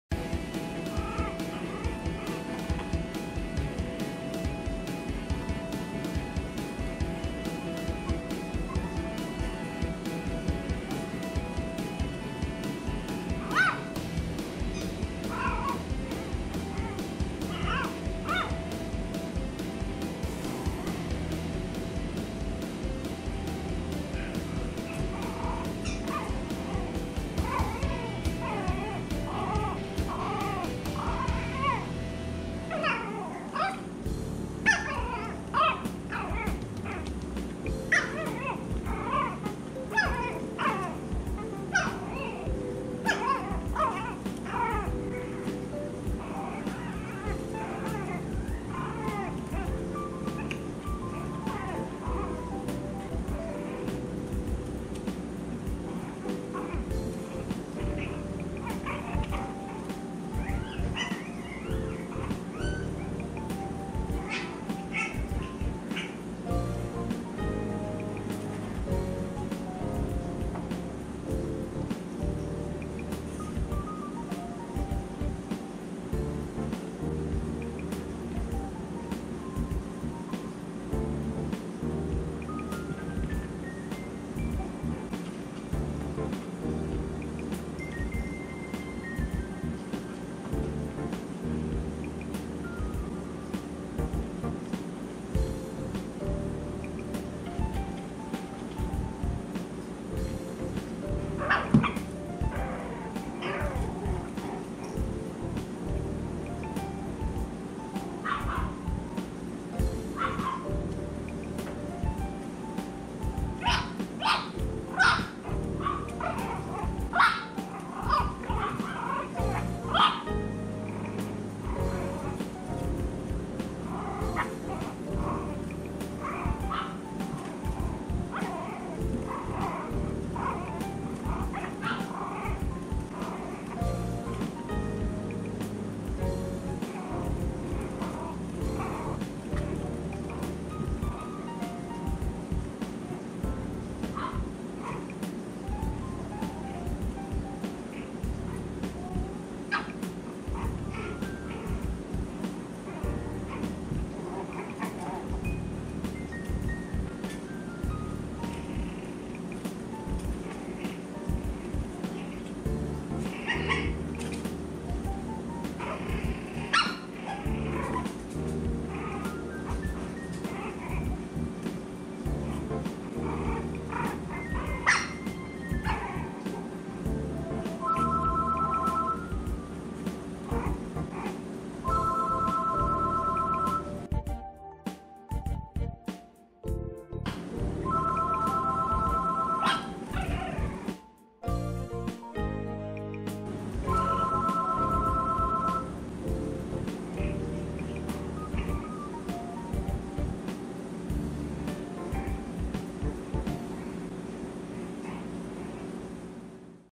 02/26/2022 Sadie's Puppies Playing on the Slide
Our puppies love their little climber slide.  They start playing with the slide during their 4th week.  They are experts by the time they reach the sixth week.